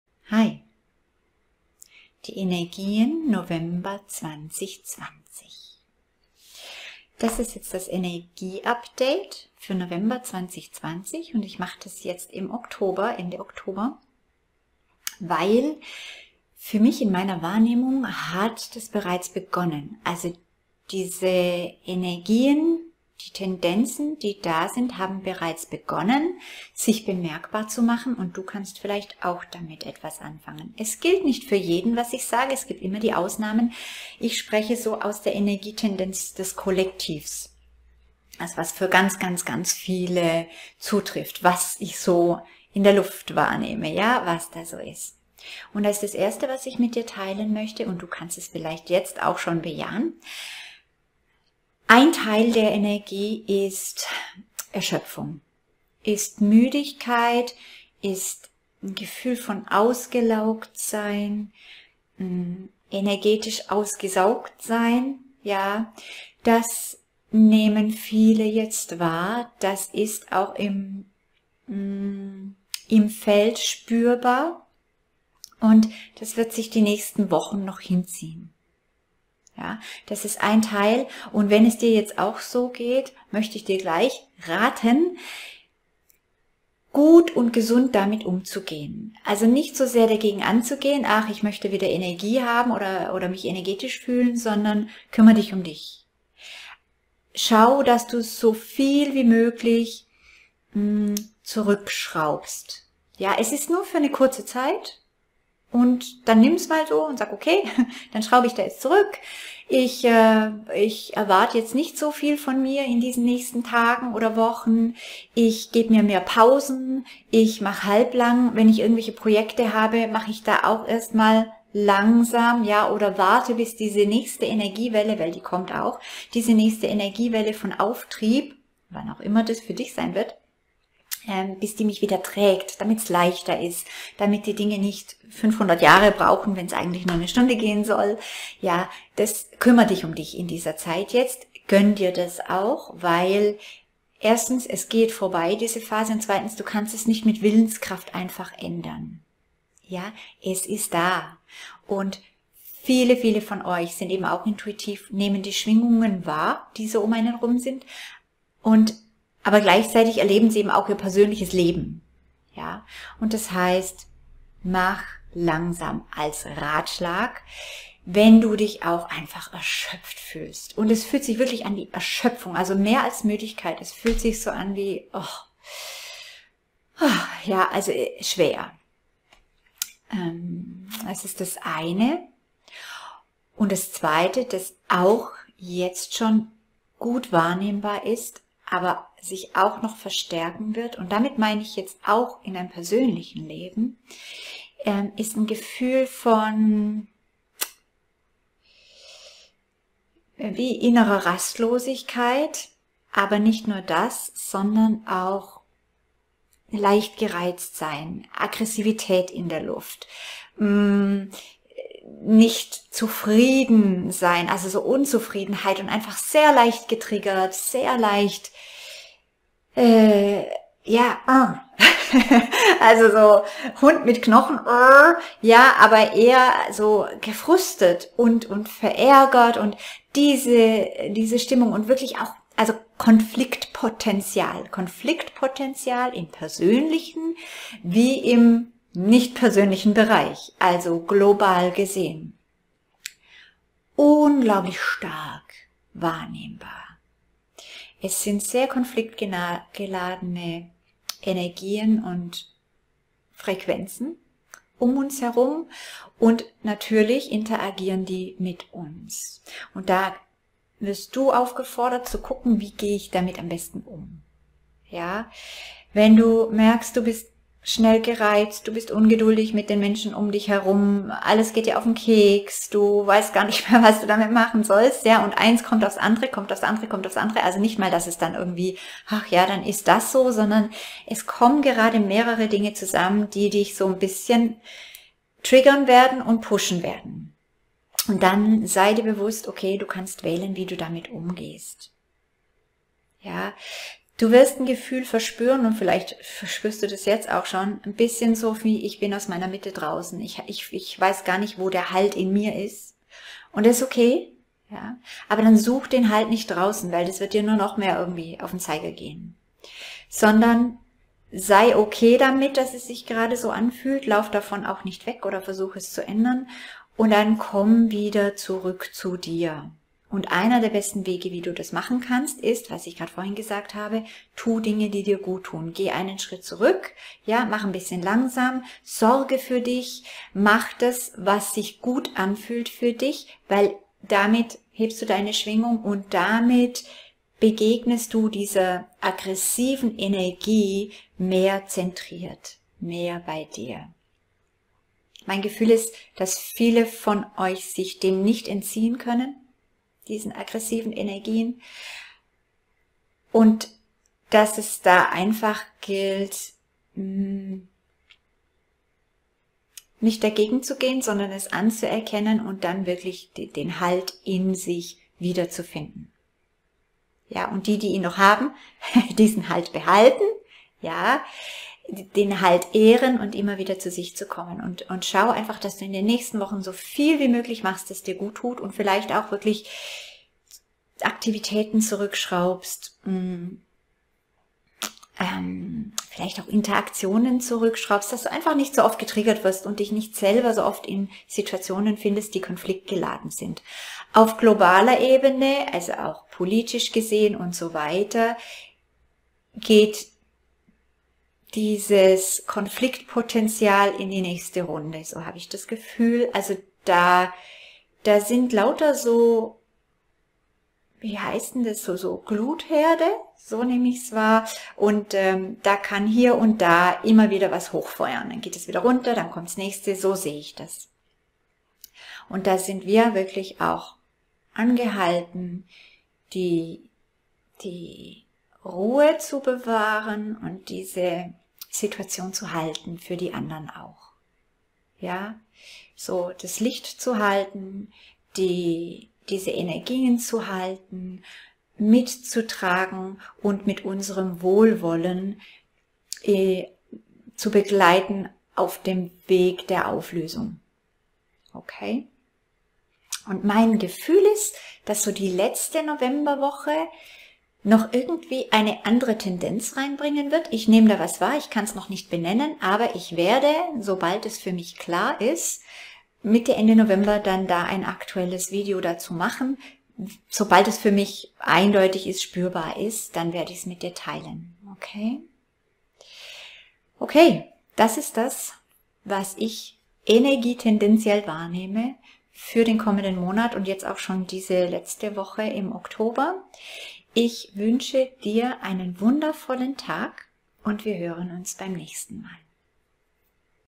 ENERGIE UPDATE NOVEMBER 2020 - AGGRESSIV und ERMÜDEND! - Mel Rentmeister
Wir haben intensive Wochen vor uns, und einige stecken schon mitten drin in dieser sehr konfliktgeladenen Energie...

Love, Mel

Herzlichen Dank!